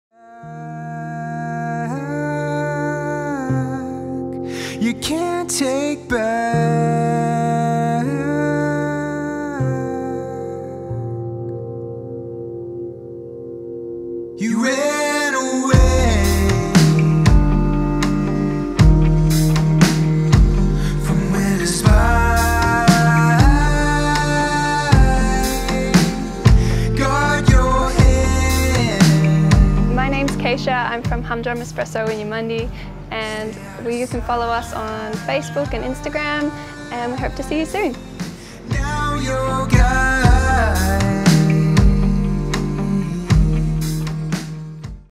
Humdrum Espresso a Cafe in Yandina QLD serving great tasting Coffee
From humble beginnings serving coffee from a cart in the park, Humdrum Espresso is now a buzzing hive of activity on Memorial Drive in Eumundi. Housed in a rustic old butcher’s shop, Humdrum exudes hipster charm with black and white themed décor, wooden tables, cool couches and an abundance of greenery and dried flowers. Local mums drop by to grab a coffee on the school run, while friends gather for a chat over a latte and croissant, all soaking up the relaxing vibe and eclectic display of collectibles. Humdrum serves up a wide variety of pastries, cakes and light lunch options such as roast veggie bagels, filled with cream cheese, zucchini, capsicum, mushrooms and caramelised onion, or pesto pasta with sundried tomato. Don’t forget to pick up a loaf of specialty bread like spelt rye.

Humdrum Espresso Cafe
100 Memorial Drive Yandina QLD Australia 4562